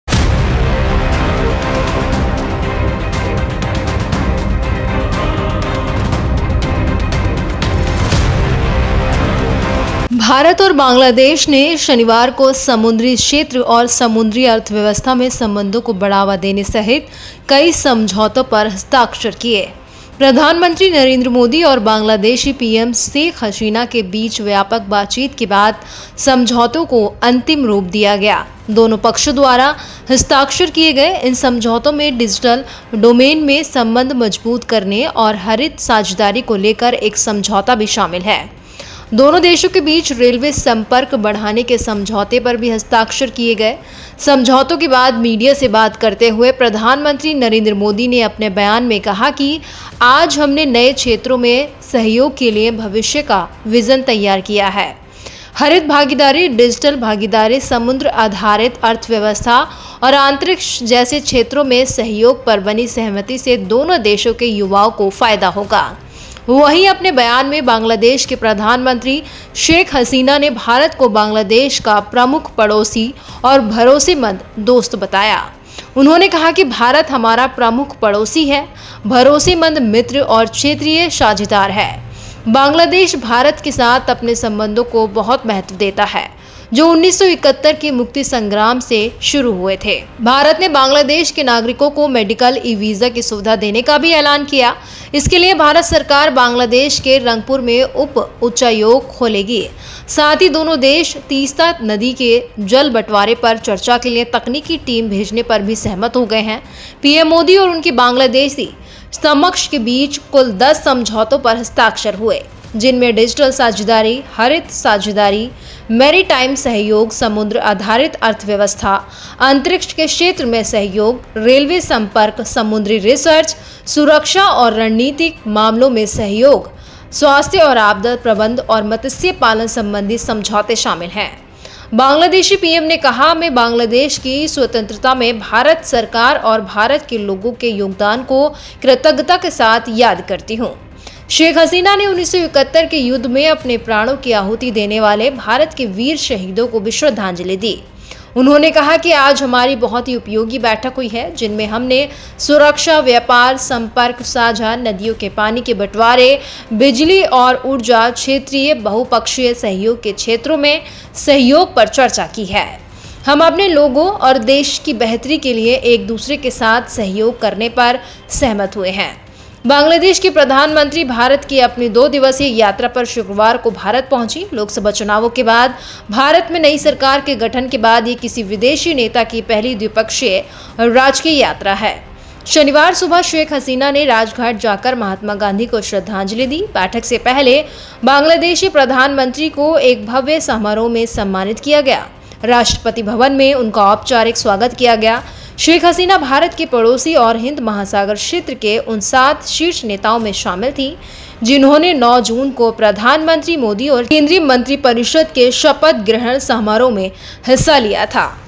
भारत-बांग्लादेश के बीच हुआ अहम समझौता | BANGLADESH | PM MODI | SHEIKH HASINA
India and Bangladesh on Saturday signed several agreements including promoting ties in the maritime sector and blue economy. The agreements were finalized after extensive talks between Prime Minister Narendra Modi and Bangladeshi PM Sheikh Hasina. The agreements signed by both sides also include an agreement on strengthening relations in the digital domain and 'Green Partnership'.